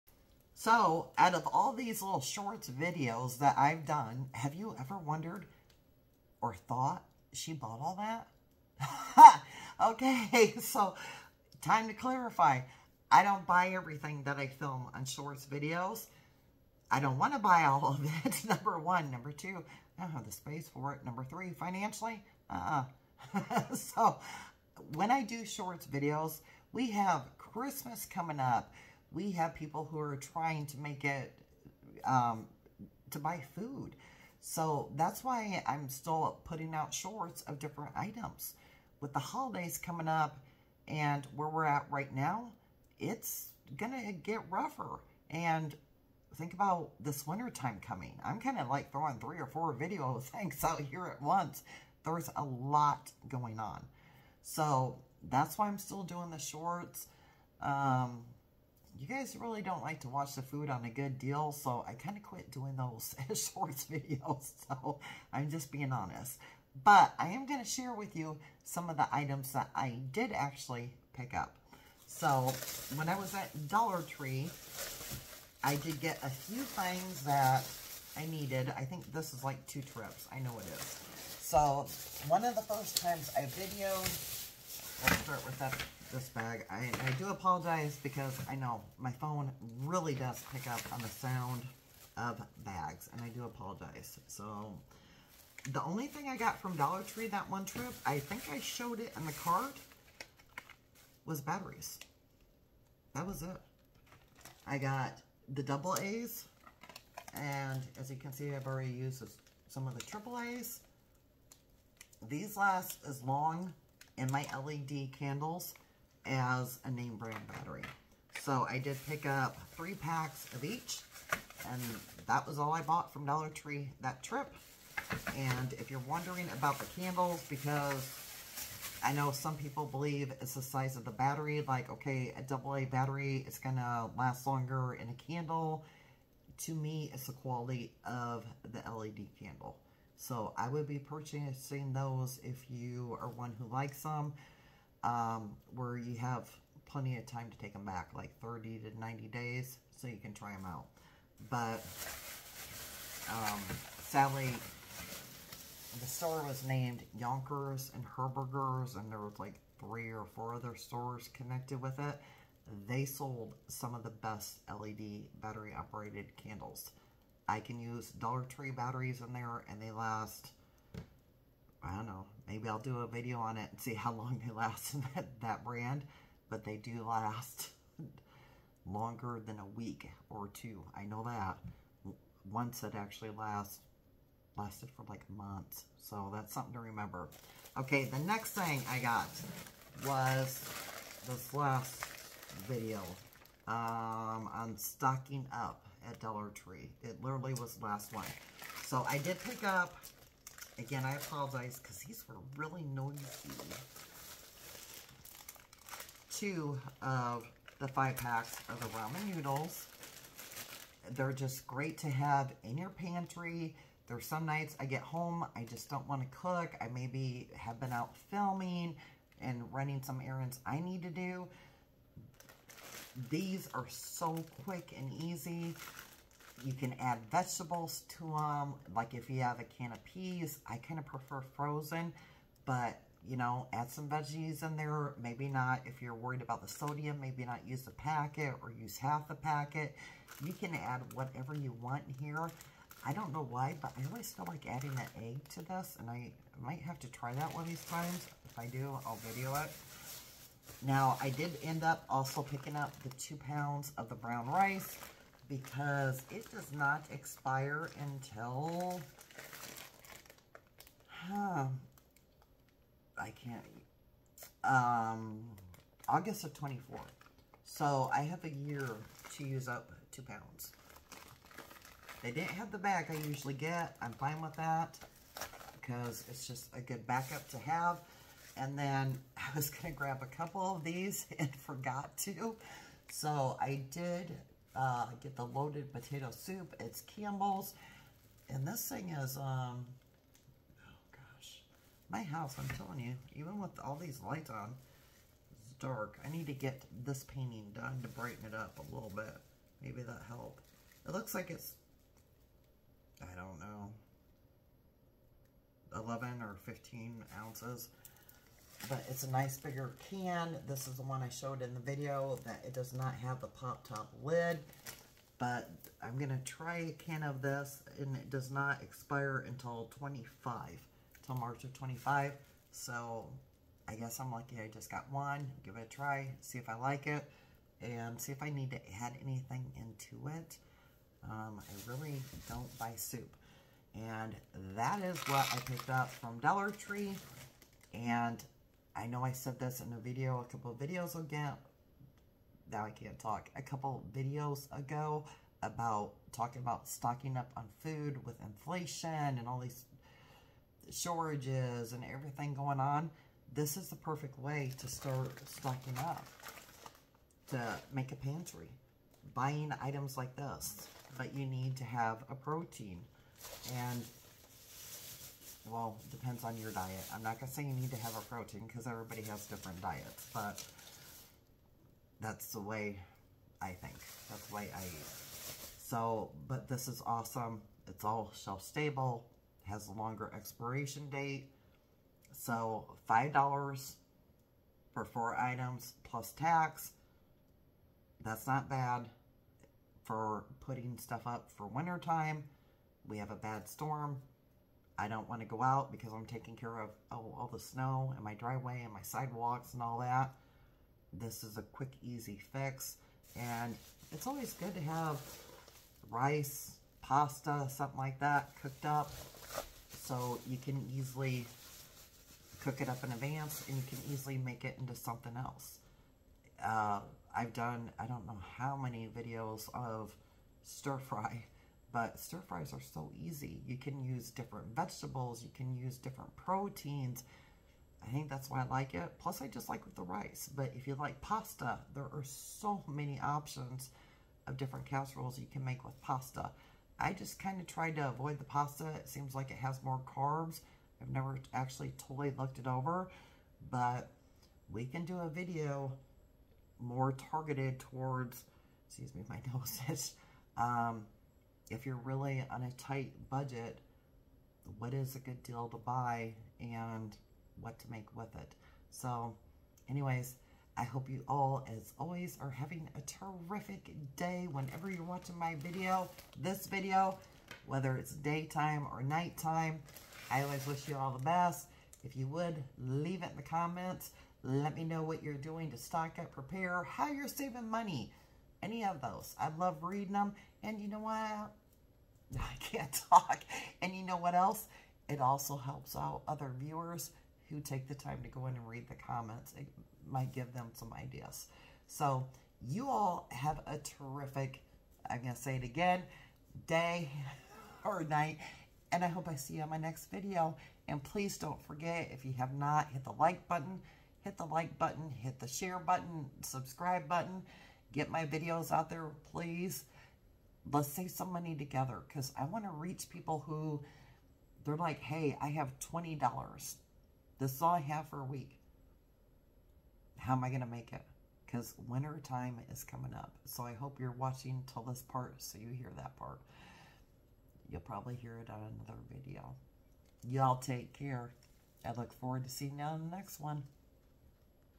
This is how I Stock Up On A BUDGET! Dollar Tree Haul!
Hi, In today's video, I'm sharing what I bought from Dollar Tree that I like to stock up on.